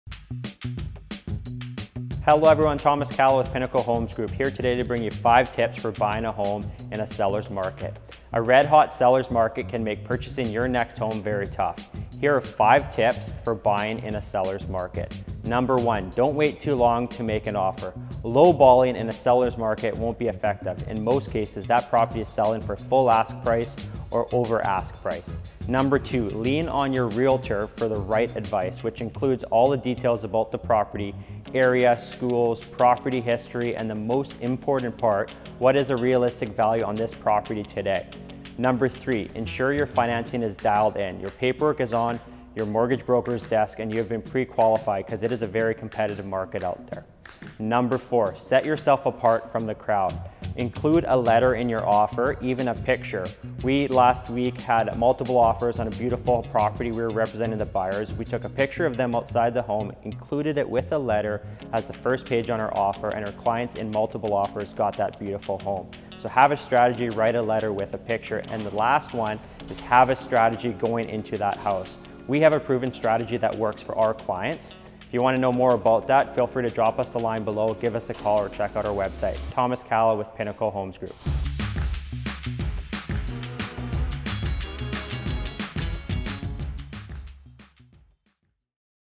5 Tips for Buying in a Sellers Market
Looking to buy a home in Victoria in 2021? Make sure to follow these 5 simple tips to help you get your dream home in a competitive sellers market! 🔽Details below 🔽

1. Don't Wait too Long to Make an Offer
2. Lean on your Realtor for Advice
3. Get your Financing in Order
4. Set Yourself Apart
5. Have a Strategy!

Looking to buy a home?
Looking to sell a home?

Thomas Kala
Personal Real Estate Corporation
Pinnacle Homes Group
eXp Realty Victoria
250-380-SOLD